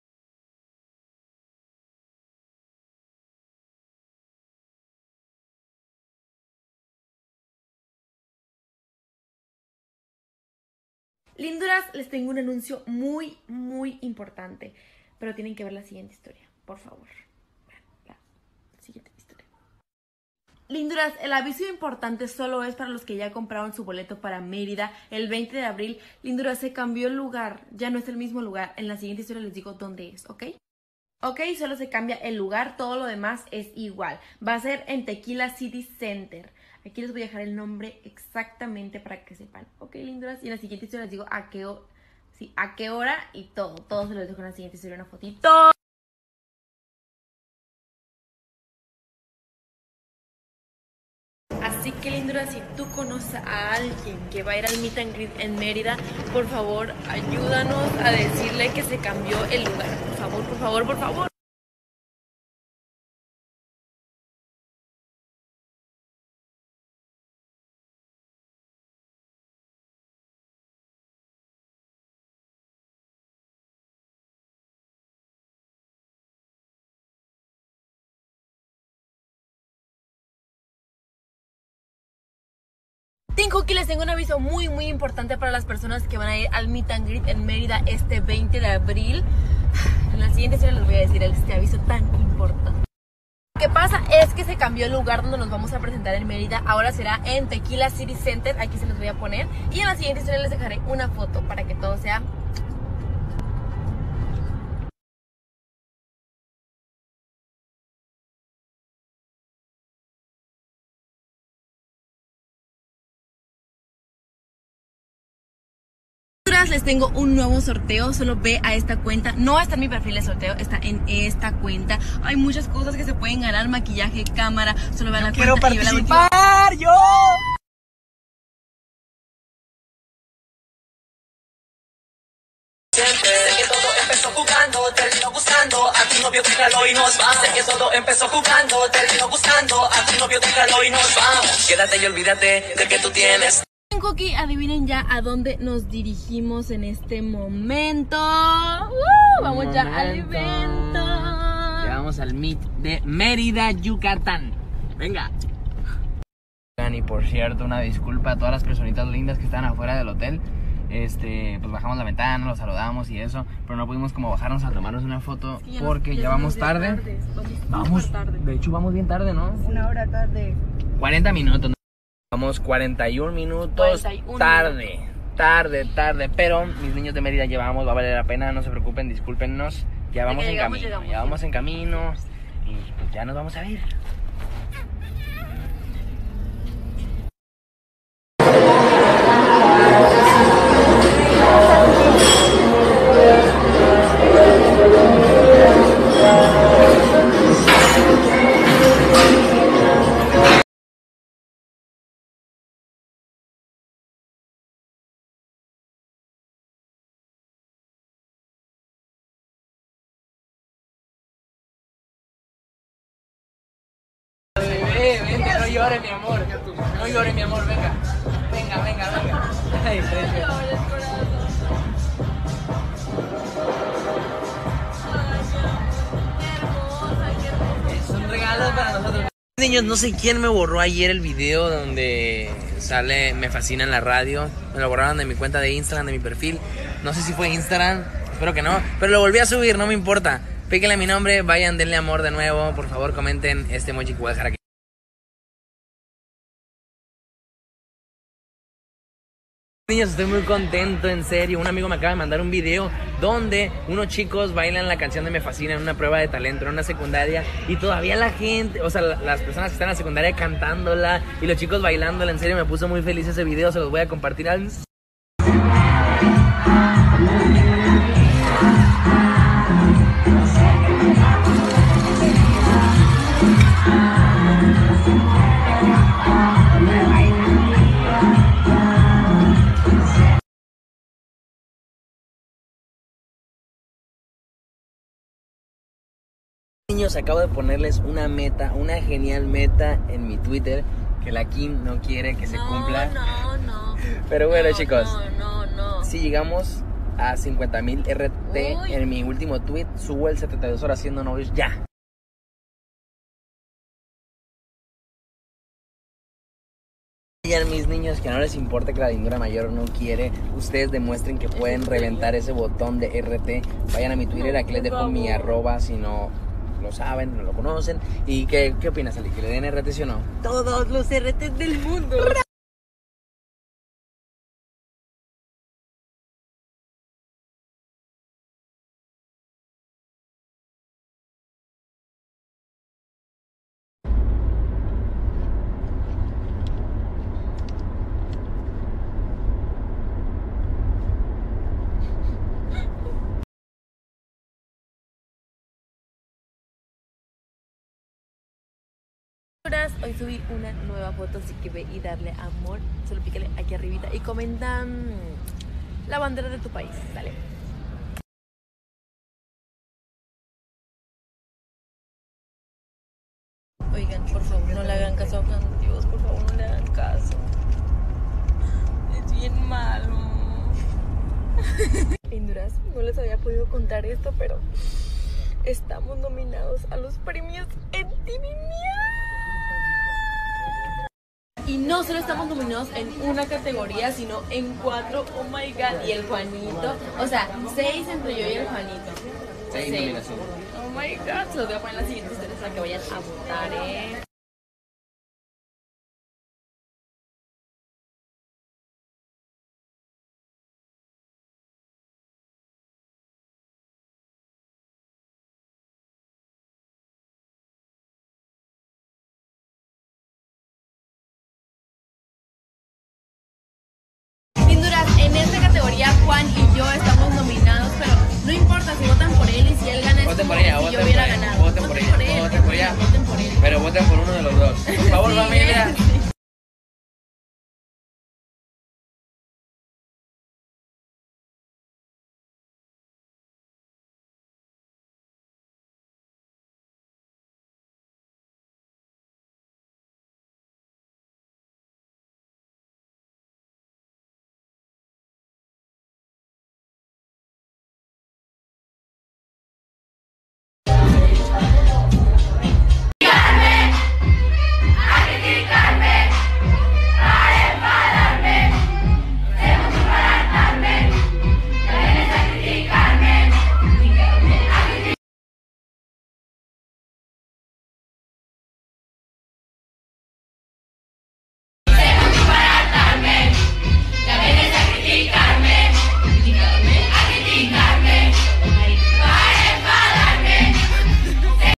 Juan De Dios Pantoja y Kimberly Loaiza compartirán el 72 horas siendo NOVIOS *Kim se NIEGA a verlo*
Muchas gracias por hacer crecer este canal, a diario nuevo contenido de tu artista o youtuber favorito.